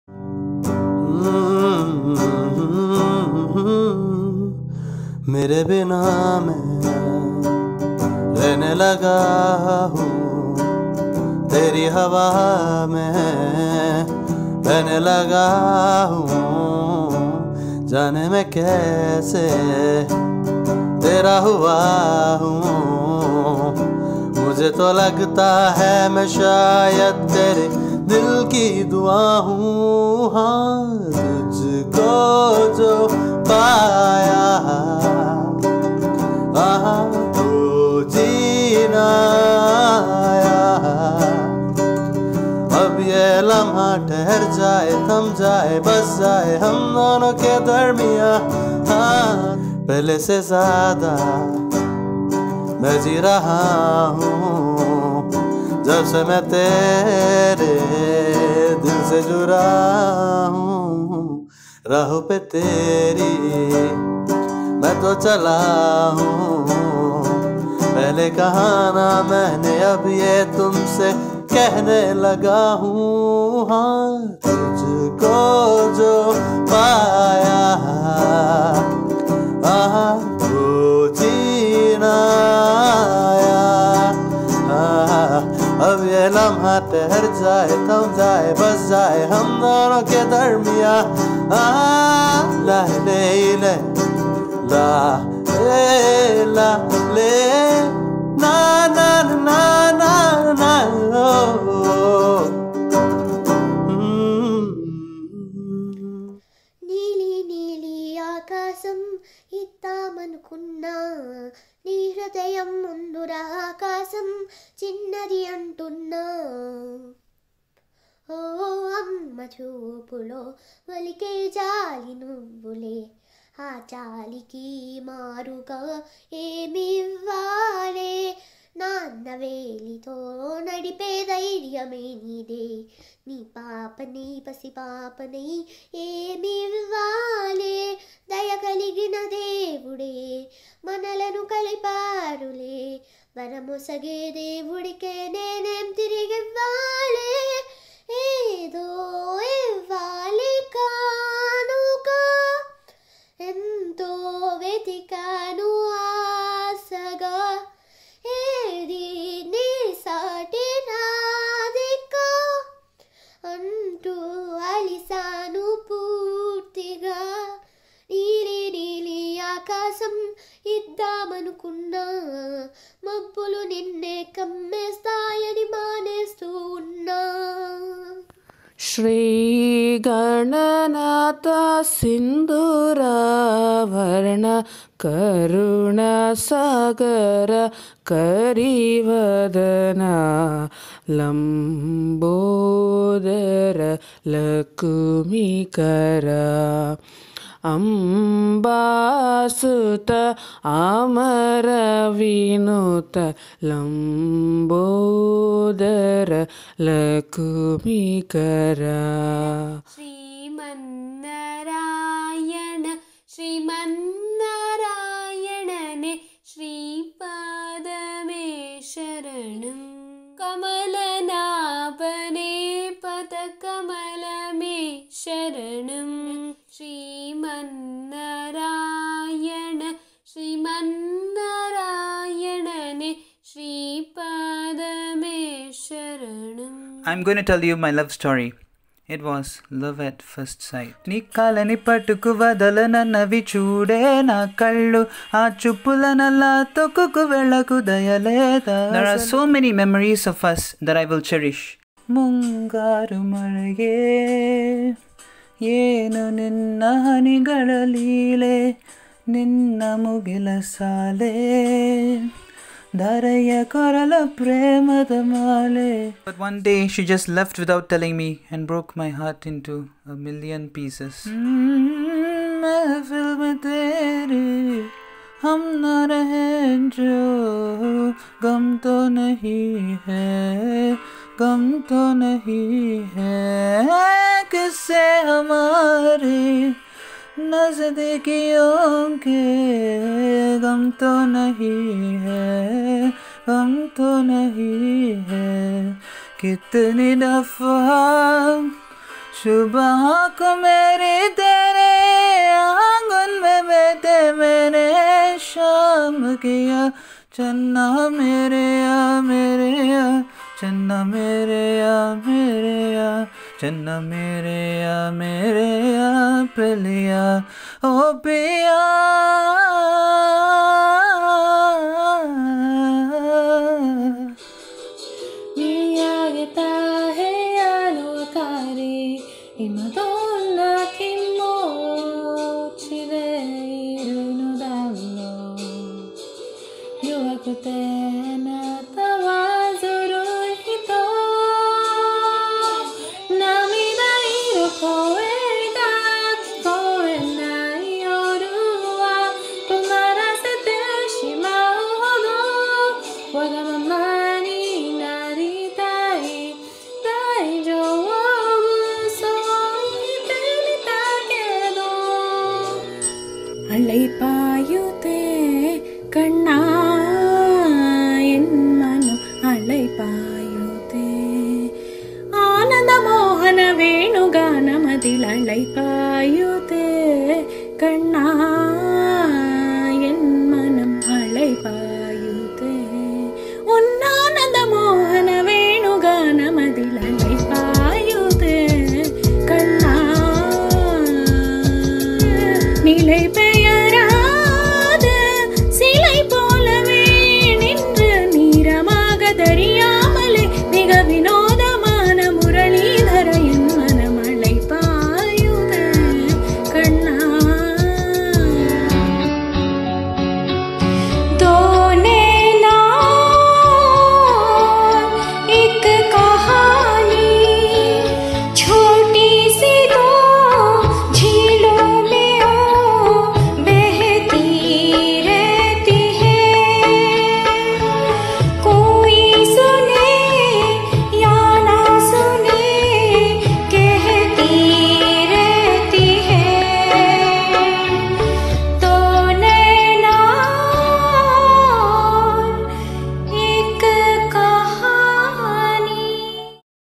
Singing Contest 2020 - Toulouse | Concours de chant | ICT
This contest was part of ICT's community engaging activity during the COVID19 situation

Ce concours faisait partie de l'activité d'engagement communautaire des TIC pendant la situation COVID19